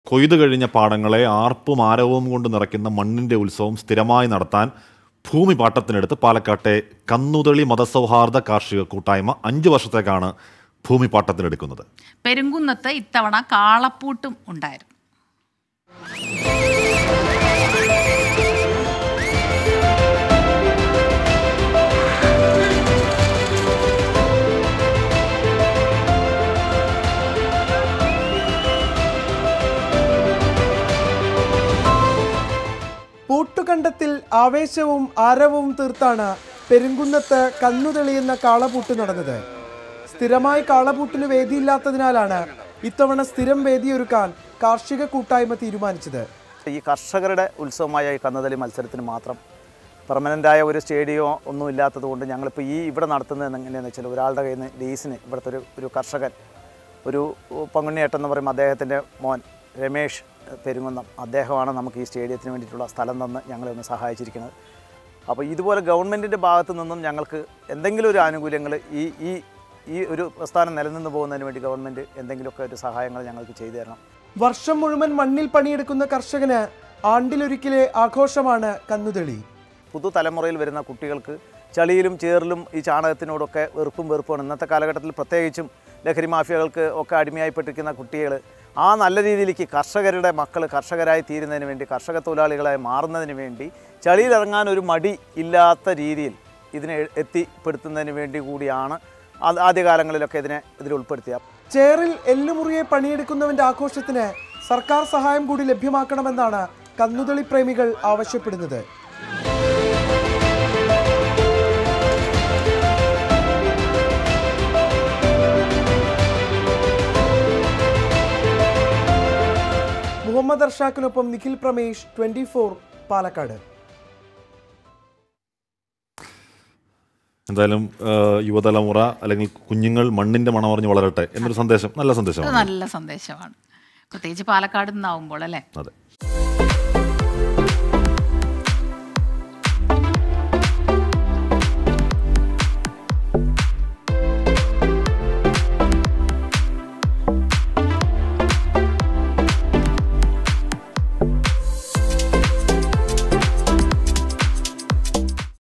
മണ്ണിന്റെയുത്സവംനടത്താന്‍ ഭൂമിപാട്ടത്തിനെടുത്ത് പാലക്കാട്ടേ കന്നുതെളി മതസൗഹാര്‍ദ്ദ കാര്‍ഷികകൂട്ടായ്മ
കൊയ്തുകഴിഞ്ഞ പാടങ്ങളെ ആര്പ്പും ആരവവും കൊണ്ട് നിറക്കുന്ന മണ്ണിന്റെയുത്സവം സ്ഥിരമായി നടത്താന് ഭൂമി പാട്ടത്തിനെടുത്ത് പാലക്കാട്ടേ കന്നുതെളി മതസൗഹാര്ദ്ദ കാര്ഷിക കൂട്ടായ്മ